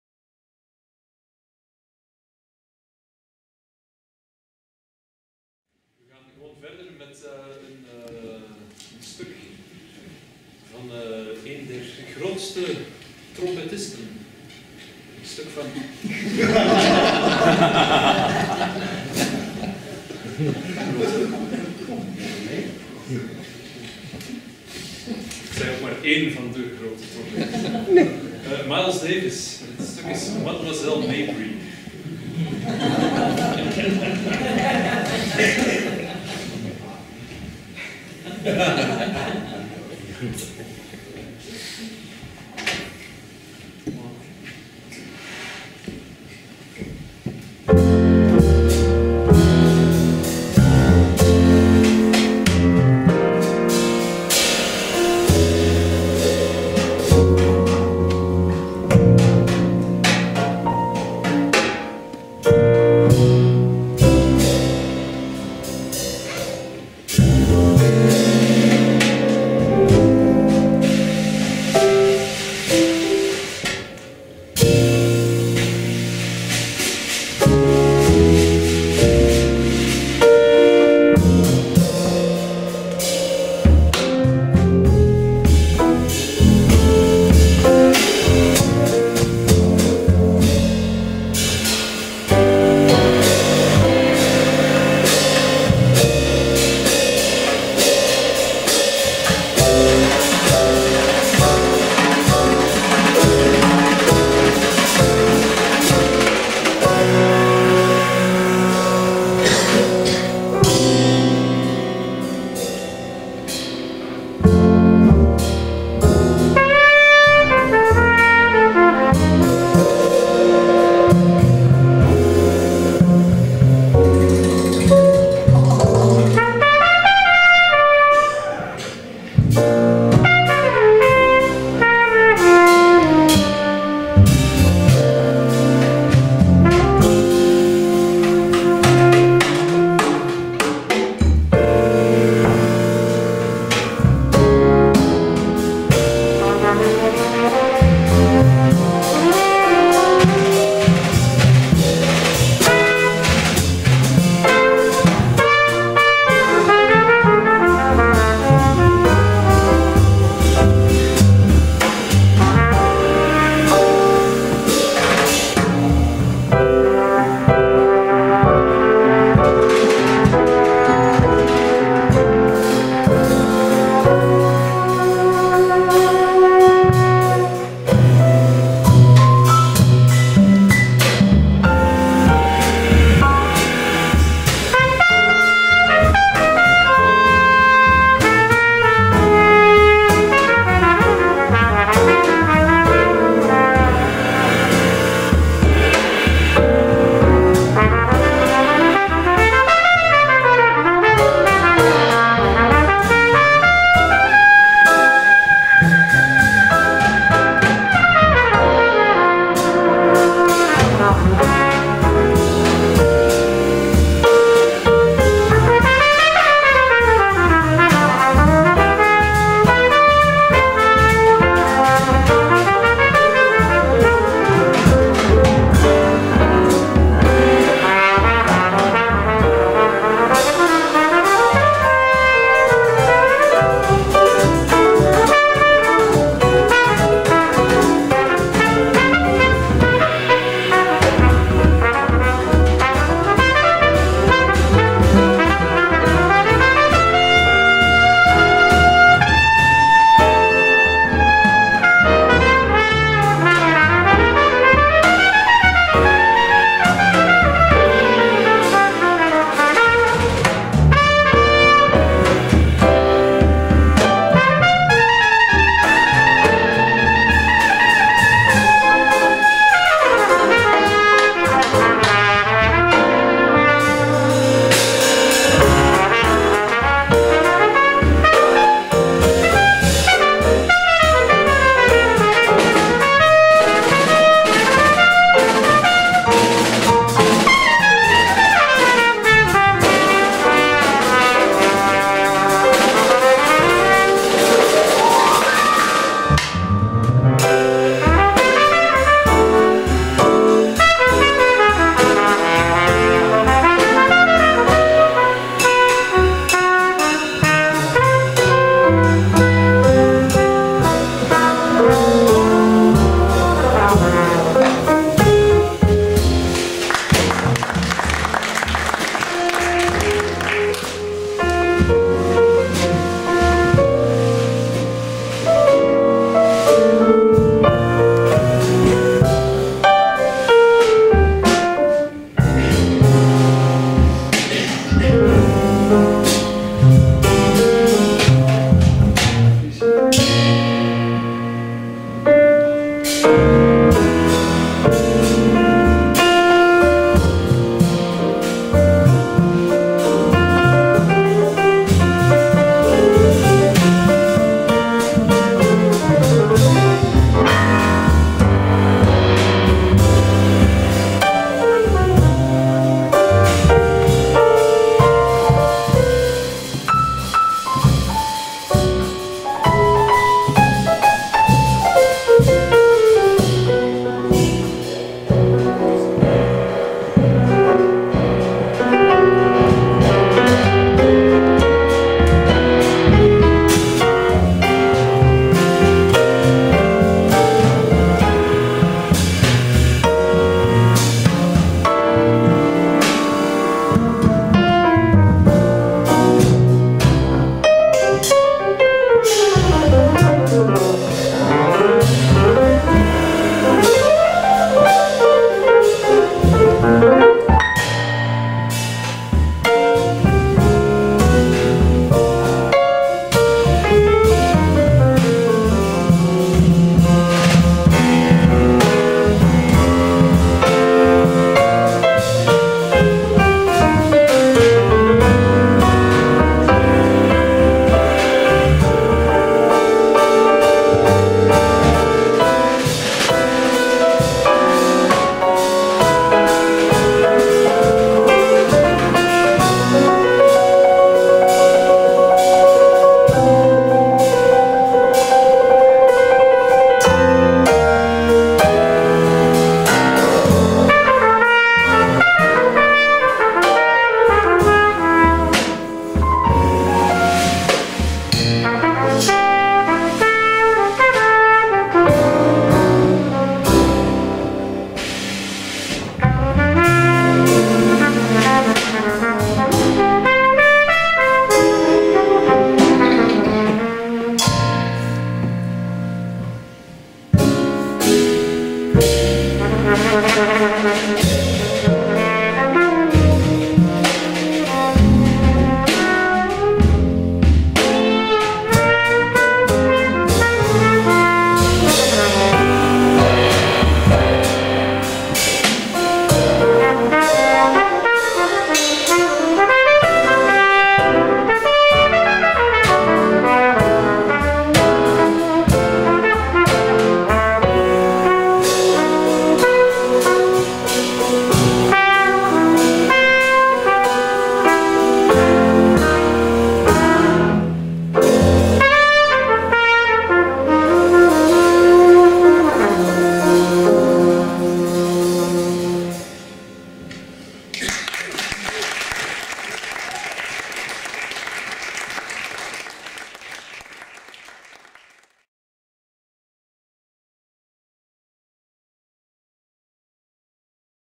Bram Weijters Quartet @ Jazzzolder (2)
10/2/2017: Bram Weijters: piano, Carlo Nardozza: trompet, Martijn Van Buel: contrabas, Lionel Beuvens: drums.